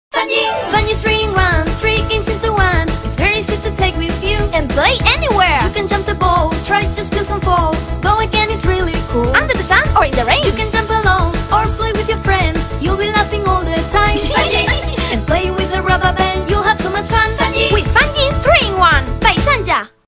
Fun Gee
FUN GEE 3 IN 1 is back! Now with MORE ATTRACTIVE LOOK with WINDOW PACKAGING!
Fun Gee, 3 Toys in 1
- You can use it as a regular JUMP ROPE or
- connect the two ends and enjoy RUBBER JUMPING with you friends!!
- But not only that!! You can also SPIN AND SKIP THE BALL